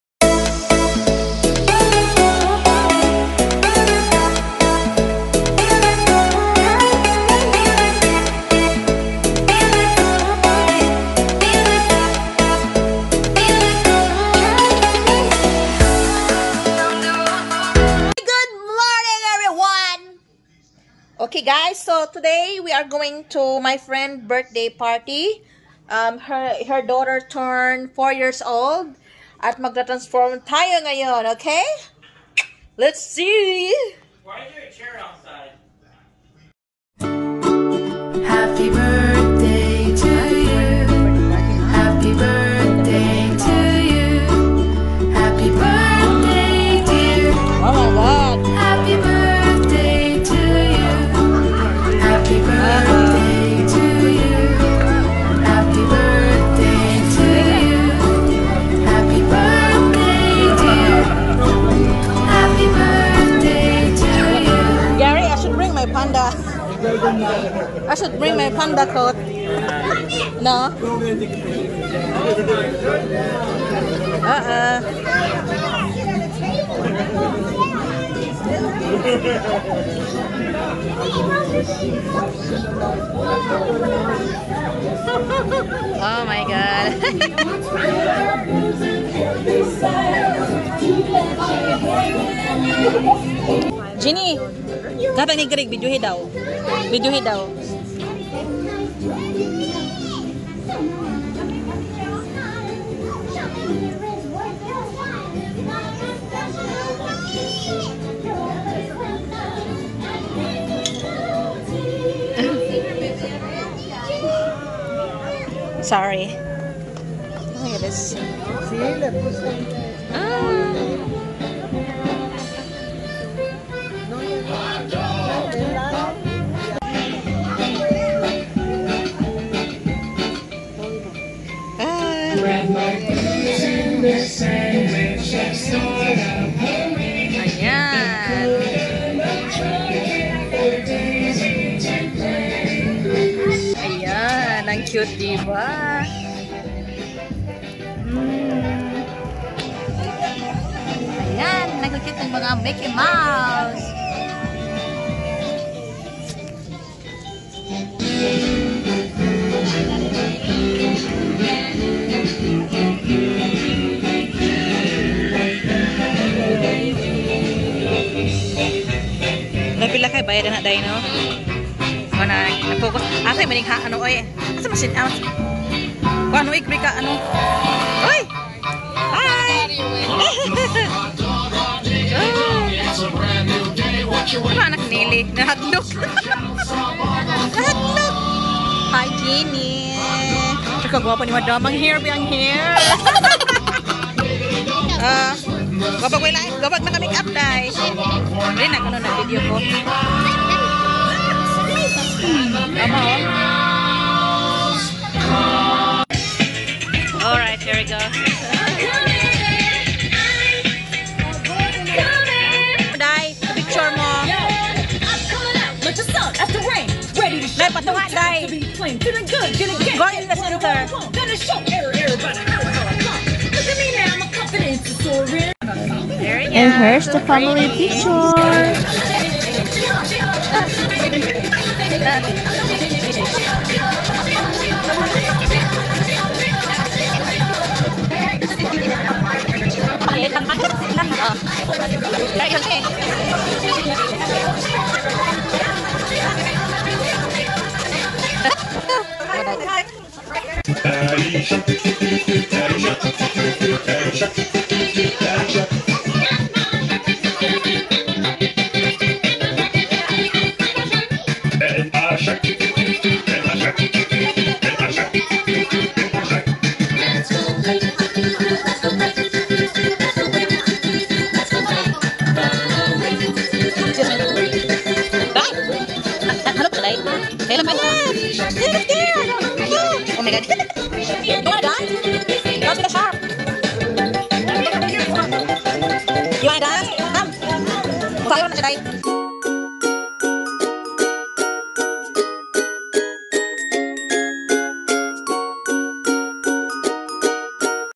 Birthday celebration with pretty Lavigne.
Attending Lavigne Birthday.
Thank you Nelly  & Mario for the invite.